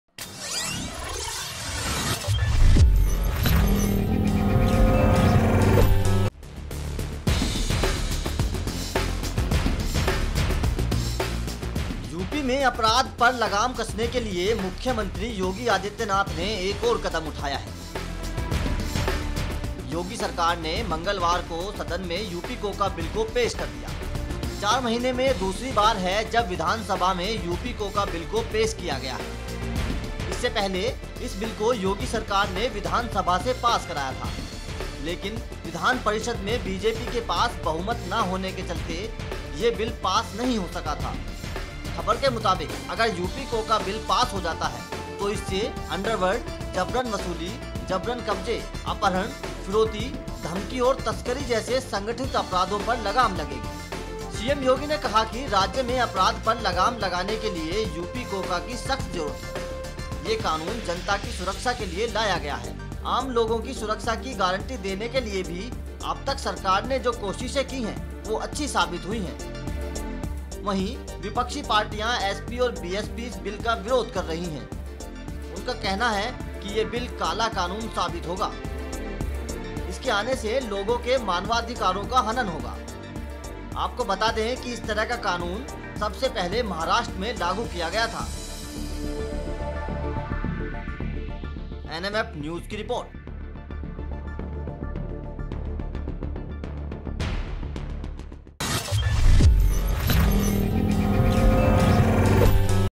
UP में Crime पर लगाम लगाएगा UPCOCA, Yogi ने Vidhansabha में पेश किया बिल
After failing to ensure the passage of the Uttar Pradesh Control of Organised Crime Bill (UPCOCA) in the Legislative Council, the Uttar Pradesh government will re-introduce it in the Legislative Assembly on Tuesday

Edit:-Pardeep Tomar